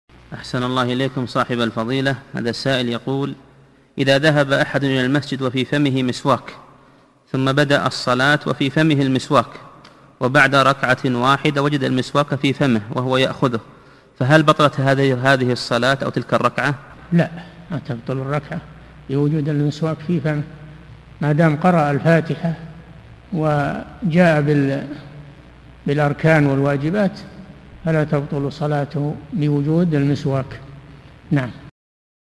[793 -858] من صلى وعلى فمه مسواك ناسيا، هل تبطل صلاته؟ - الشيخ صالح الفوزان
"السؤال: إذا ذهب أحد إلى المسجد وفي فمه مسواك ثم بدأ الصلاة وفي فمه المسواك وبعد ركعة واحدة وجد المسواك في فمه وهو يأخذه فهل بطلت هذه الصلاة أو تلك الركعة؟
الجواب: لا، ما تبطل الركعة لوجود المسواك في الفم، ما دام قرأ الفاتحة وجاء بالأركان والواجبات فلا تبطل صلاته بوجود المسواك.

للاشتراك في قناة الشيخ صالح الفوزان

للاستفسار والإقتراحات والدعم
رئيس الفريق
لمشروع كبار العلماء